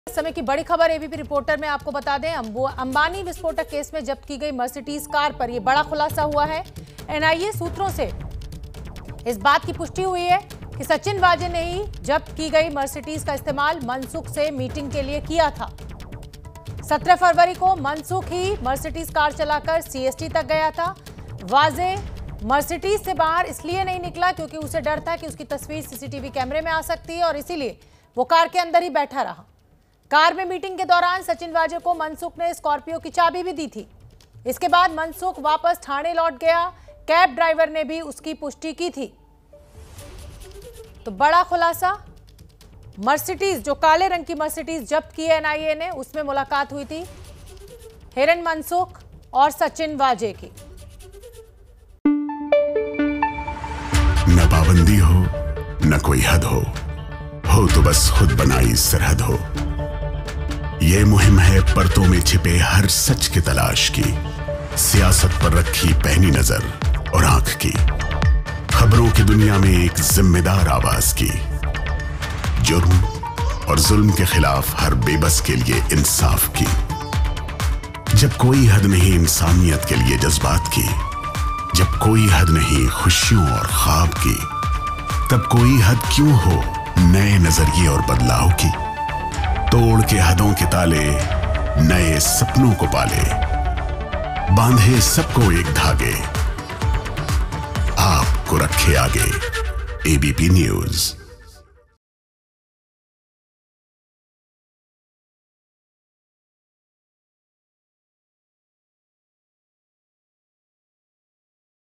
Mukesh Ambani Antilia Case में जब्त मर्सिडीज को लेकर बड़ा खुलासा, Vaze ने किया था कार का इस्तेमाल
एंटीलिया विस्फोटक केस में जब्त मर्सिडीज को लेकर बड़ा खुलासा हुआ है. सूत्रों के हवाले से खबर है कि मनसुख हिरेन से मीटिंग के लिए सचिन वाजे ने मर्सिडीज कार का इस्तेमाल किया था.